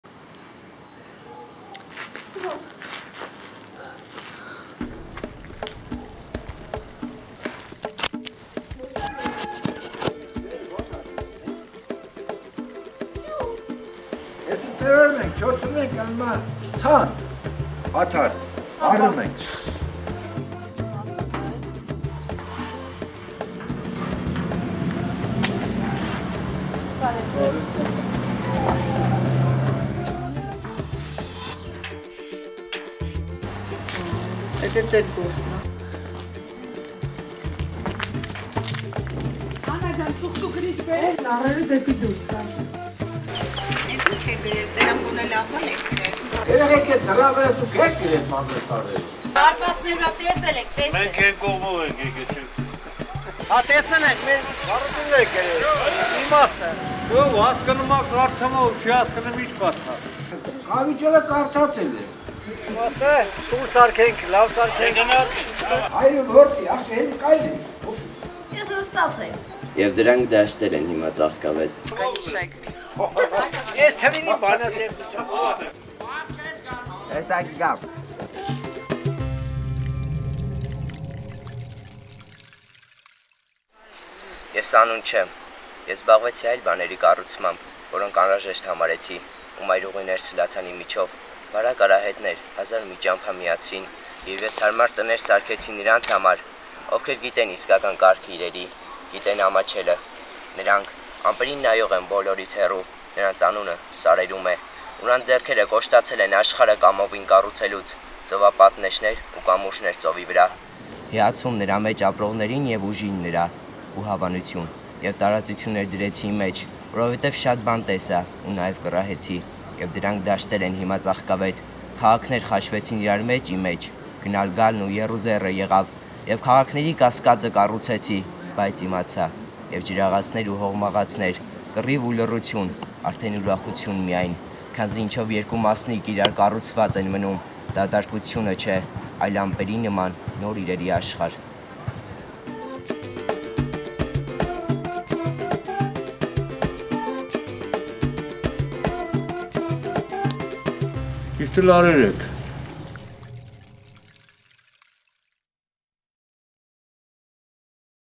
Anna Davtian "Book of Gratitude" - I am not a Name
Ես անուն չեմ.
ես զբաղվեցի այլ բաների կառուցմամբ,
որոնք անհրաժեշտ համարեցի,
ու մայրուղիներ սլացան իմ միջով,
բարակ արահետներ հազար ու մի ճամփա միացրին,
եւ ես հարմար տներ սարքեցի նրանց համար, ովքեր
գիտեն իսկական կարգը իրերի,
գիտեն ամաչելը.
նրանք
ամպերին նայող են բոլորից հեռու,
նրանց անունը սարերում է,
ու նրանց ձեռքերը կոշտացել են աշխարհը կամովին կառուցելուց`
ծովապատնեշներ ու կամուրջներ ծովի վրա,
հիացում նրա մեջ ապրողներին եւ ուժին նրա,
ու հավանություն:
Եւ տարածություններ դրեցի իմ մեջ, որովհետեւ շատ բան տեսա
ու նաեւ կռահեցի,
եւ դրանք դաշտեր են հիմա ծաղկավետ:
Քաղաքներ խաչվեցին իրար մեջ իմ մեջ,
գնալգալն ու եռուզեռը եղավ,
եւ քաղաքների կասկածը կառուցեցի,
բայց իմացա:
Եւ ջրաղացներ ու հողմաղացներ -
կռիվ ու լռություն,
արդեն ուրախություն միայն -
քանզի ինչով երկու մասնիկ իրար կառուցված են մնում,
դատարկությունը չէ,
այլ ամպերի նման` նոր իրերի աշխարհ:

I am not a name,
I busied myself building other things,
which I found necessary,
and highways rose up inside me,
narrow footpaths bridged a thousand roads,
and I built cozy houses for those,
who know the true order of things,
those
who know shame,
those solitary cloud-watchers,
whose names are in the mountains,
and whose hands are hard from the willing-work
of making the world -
seawalls and bridges over the sea,
admiration for its dwellers and its strength,
and approval.
I placed expanses in me, since I had seen much and guessed also,
and they are flowery fields of mine now.
Cities crossed each other in me,
there whirled the traffic and the wind,
and I built their urban doubt,
but I discovered.
Both watermills and windmills -
battle and silence,
only joy now -
for what two particles remain built together with
is not emptiness,
but like clouds, a new world of objects.